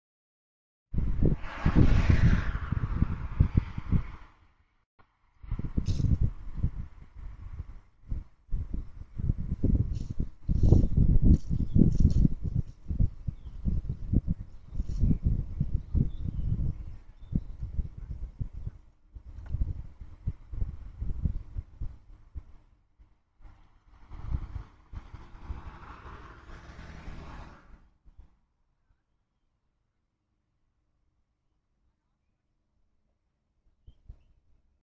crack head digging my neighbors garbage 🗑
Crack Head caught stealing mail